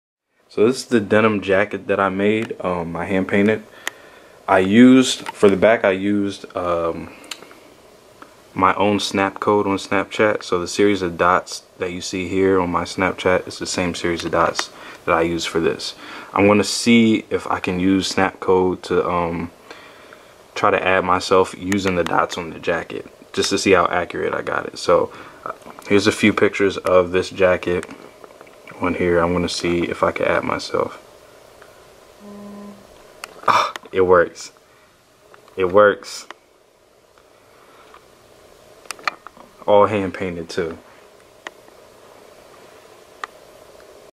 " I PAINTED MY SNAP CHAT CODE ON A JACKET...AND IT WORKS!"
I Painted my snap code on the back of a denim jacket. After taking a picture of its and using the Snapchat app to scan the code, I discovered that my hand painted artwork was detectable. Now that's what I call accuracy.